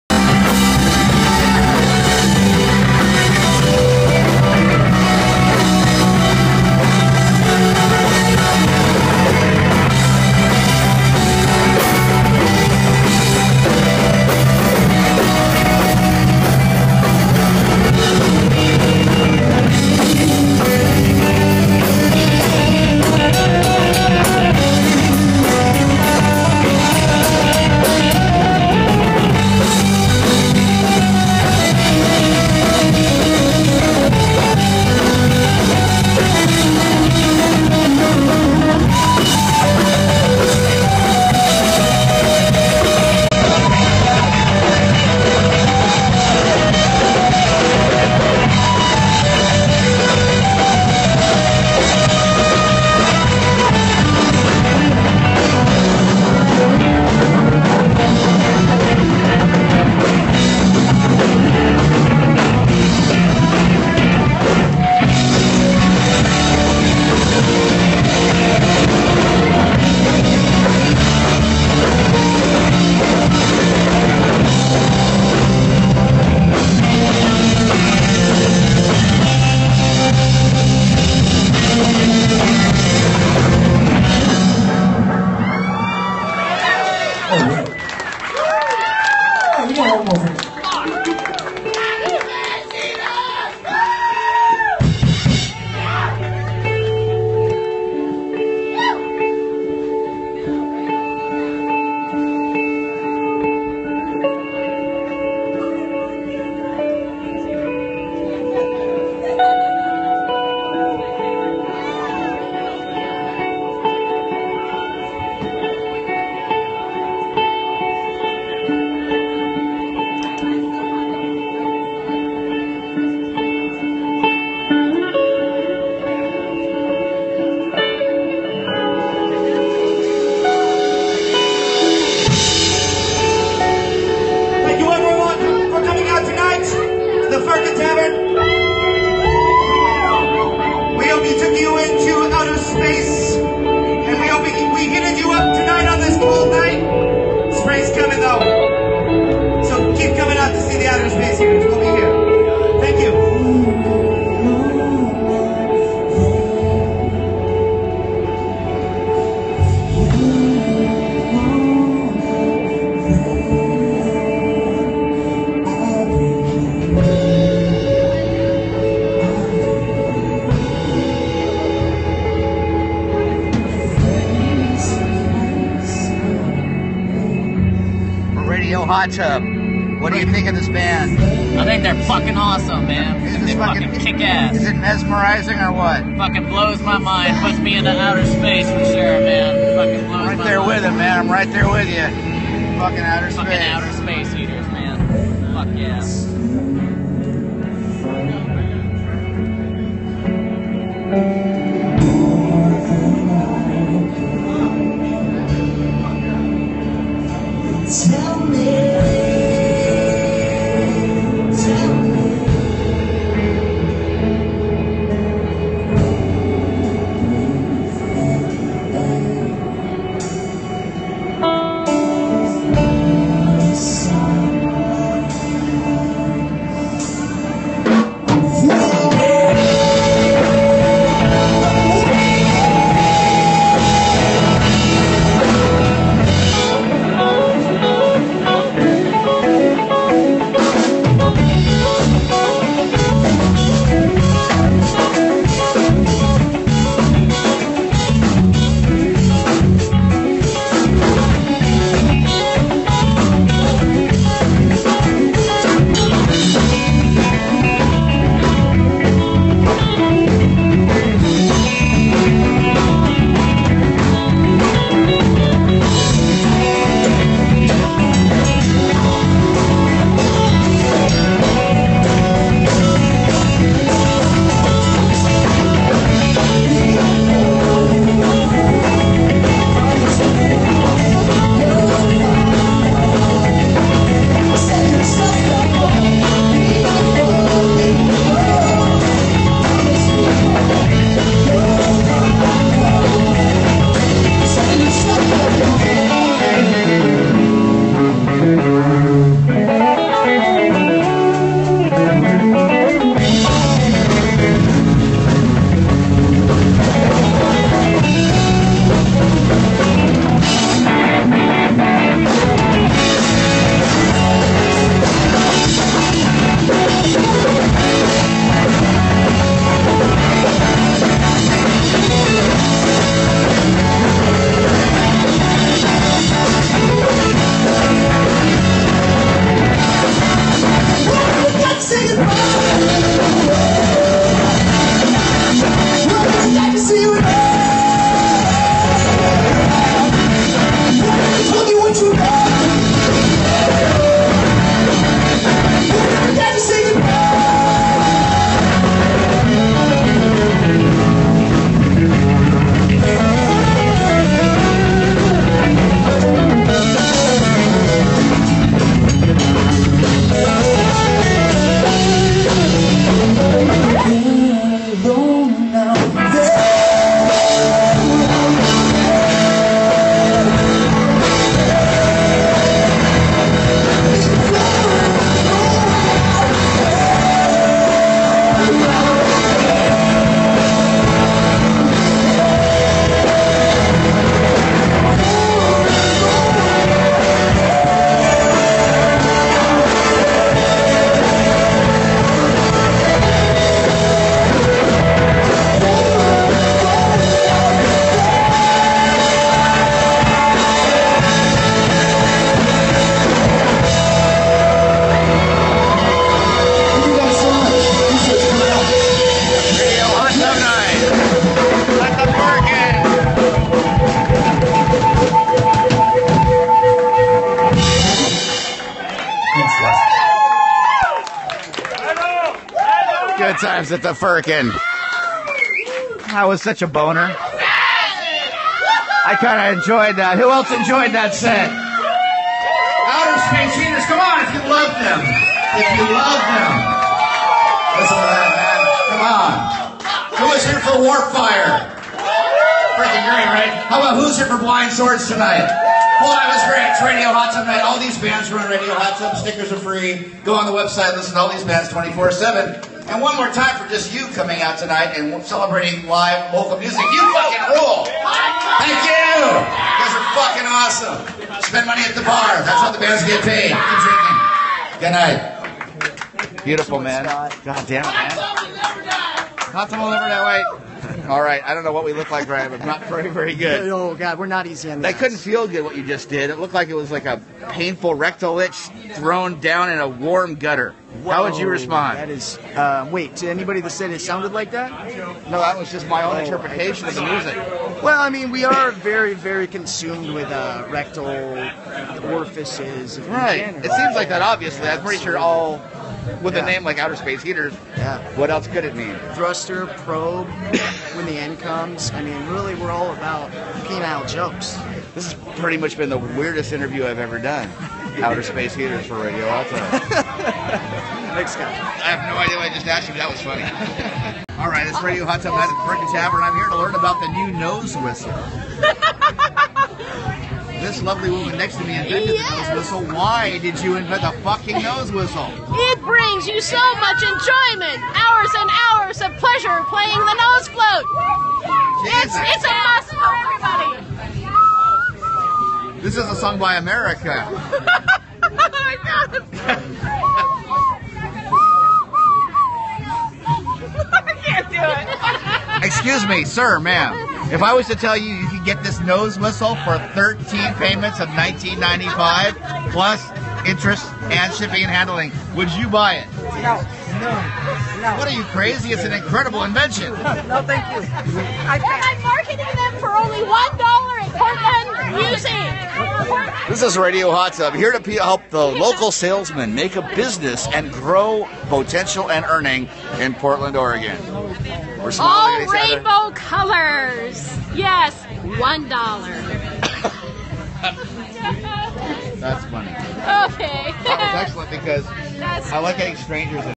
Radio Hot tub Night/ Outer Space Heaters
Outer Space Heaters rock The Firkin Tavern on Radio Hot tub Night 2/27/2016